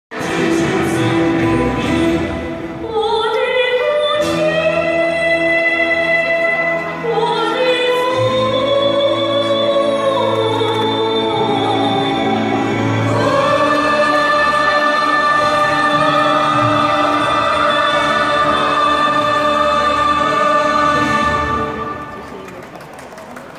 Changchun university in china rehearsal for Anniversary
Students life in changchun  china